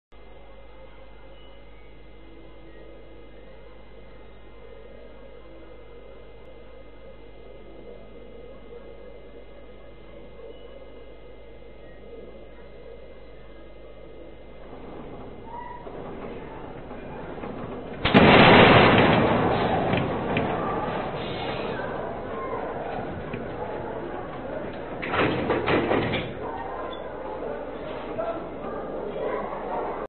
Gilletin Stick Blast in Shimoga
Recently blast of Gilletin Sticks in Shimoga Creates Earthquake in Shimoga